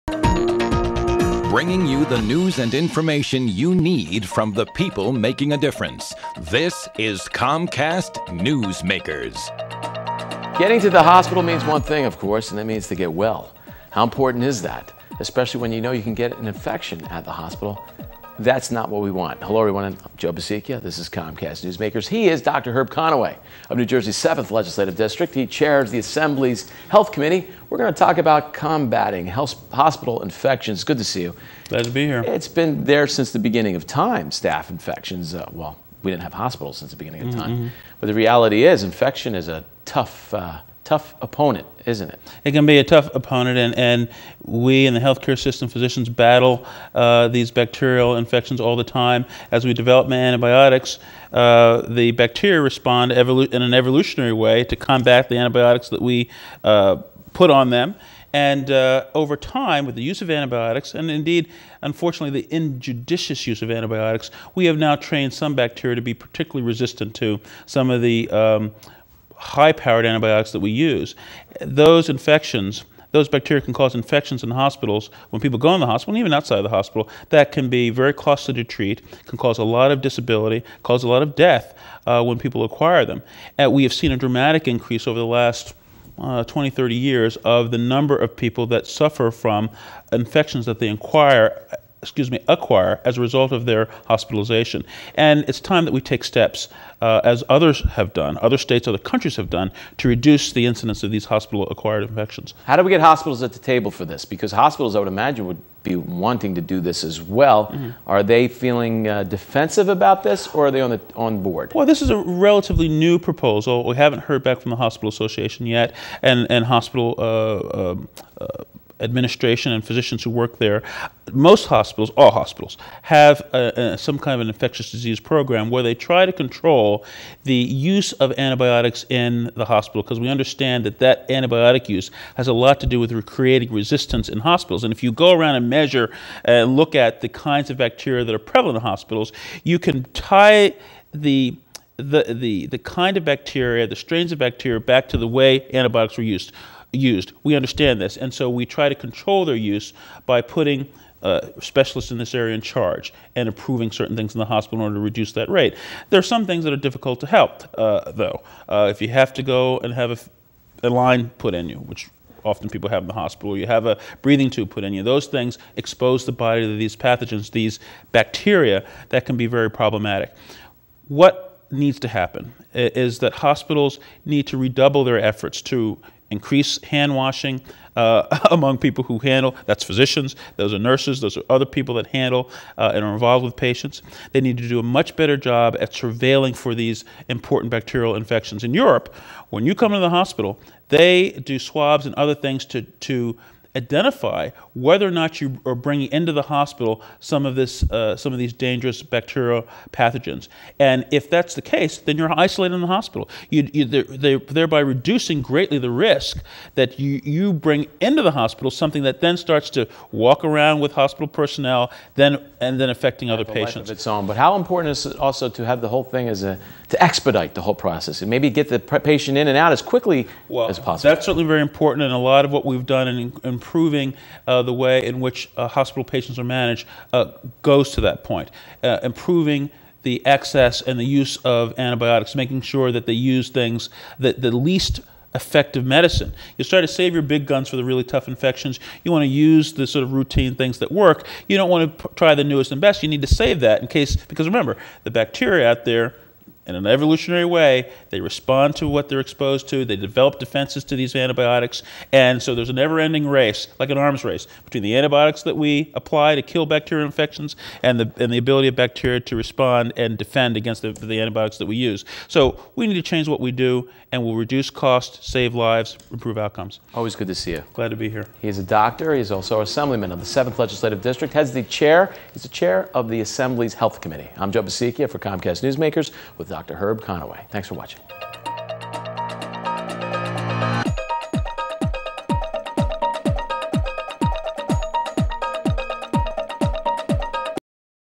Conaway on combatting hospital infections
Assemblyman Herb Conaway, MD talks about steps being taken to combat hospital infections in New Jersey.
Comcast Newsmakers - May 8, 2007